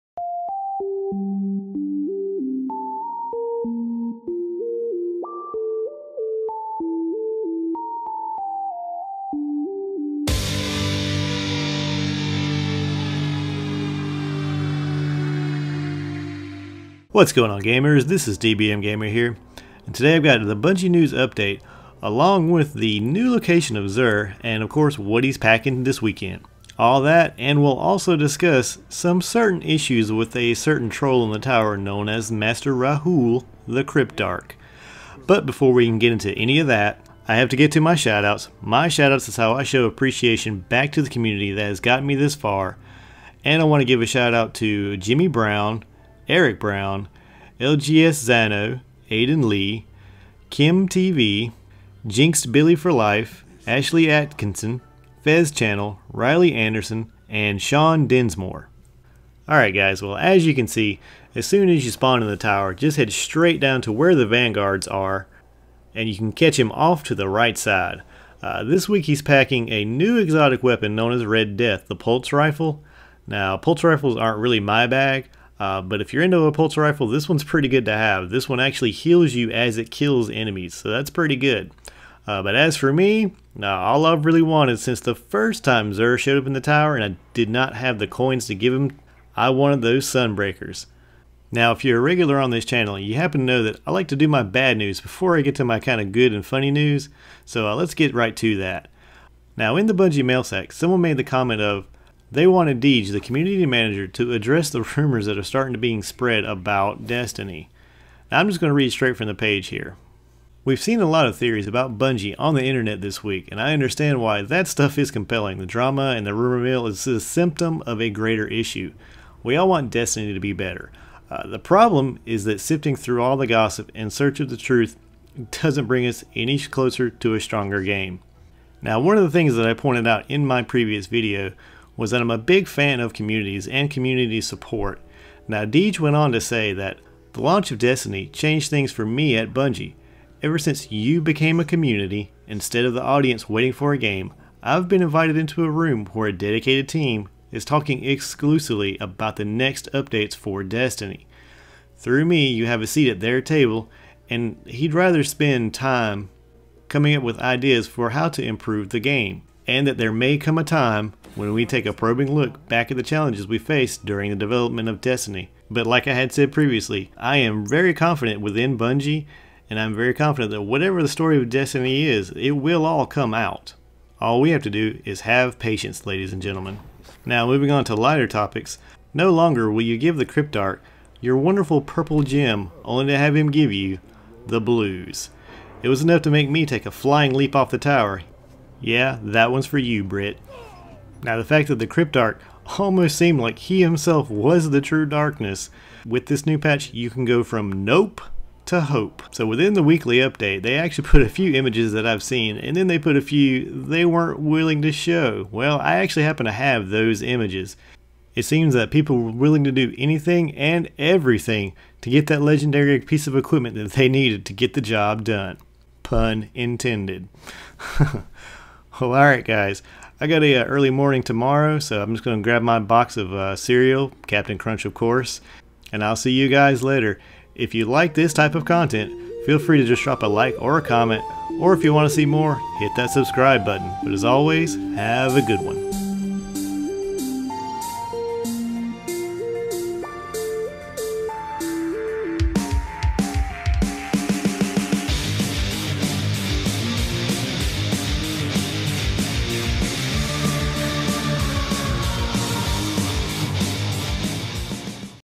Destiny News Update - Rumors and Trolls
A quick news update of Xur's new location and what he is packing this weekend.  Also I cover Bungie's Response to the recent Rumors going around.  All that a few Lolz too!